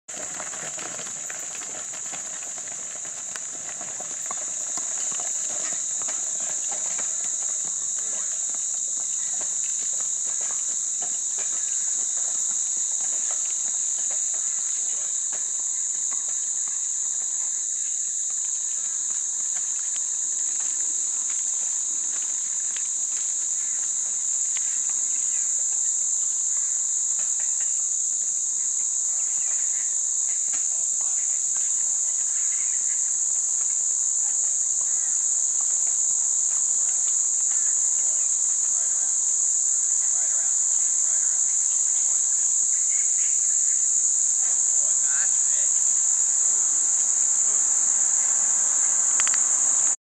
Chubbs and Spencer in Aussie forecart
This is another video of Chubbs and Spencer in the newly finished "Outback forecart".  Great Success.  So awesome to finally have it up and going.  Now all we need to do is get the implements going.  Guess it is a start and I don't know where you can buy anything like this in Australia so we are pretty excited